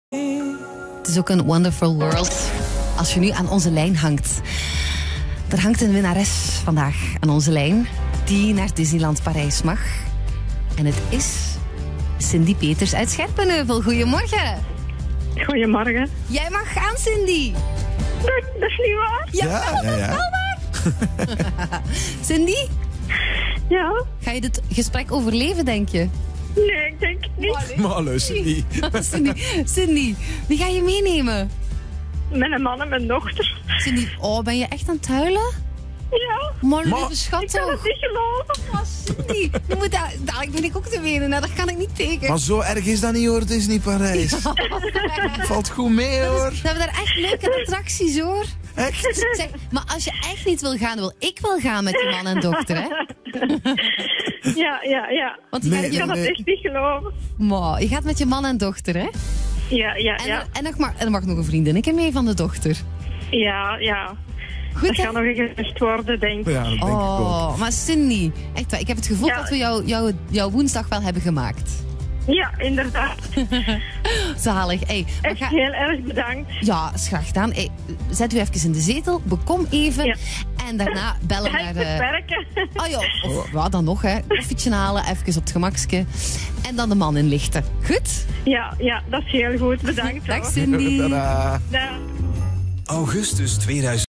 Sven & Anke ontroerd door Disneywinnares Cindy
Cindy uit Scherpenheuvel was vanmorgen de winnares van een verblijf naar Disneyland Parijs. Haar reactie was ontroerend!